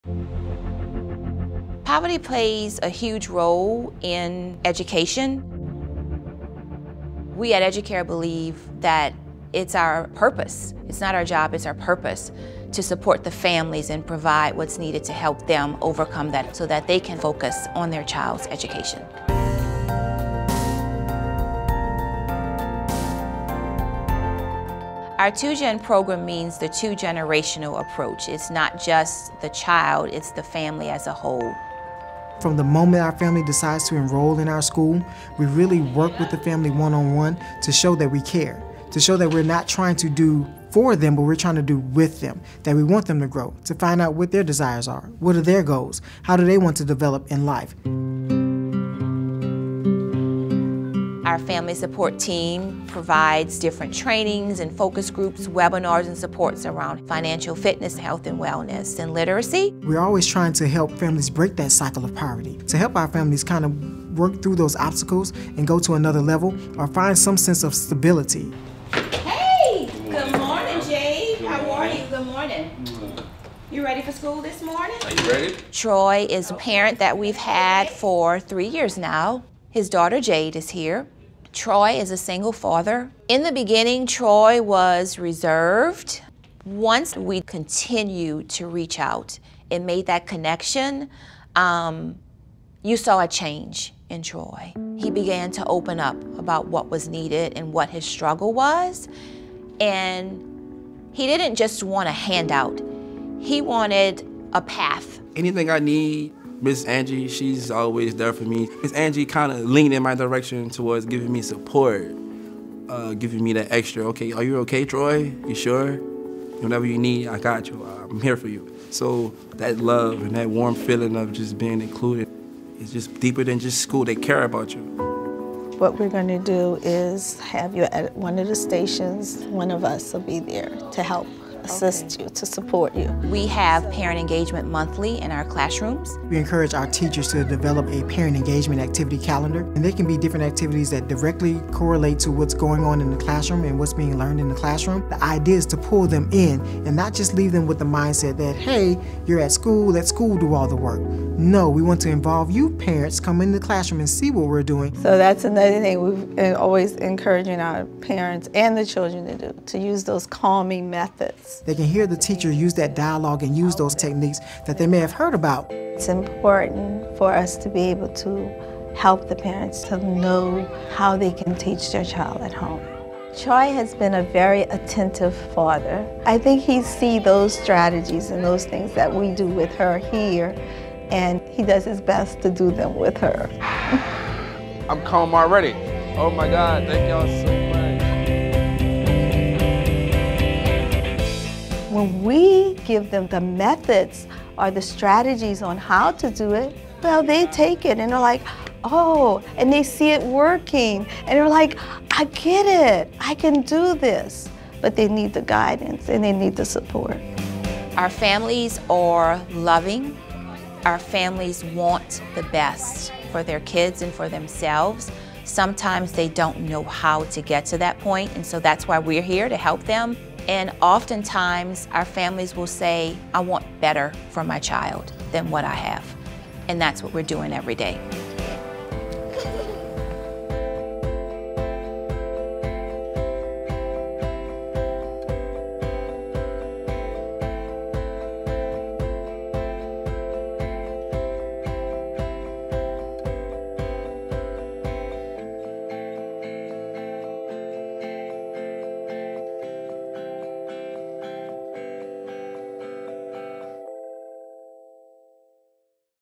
Supporting Families to Boost Students’ Success
Addressing the whole family’s financial, social, and emotional wellness supports their child’s progress in school.

EduCare New Orleans
GRADE Pre-K | New Orleans, LA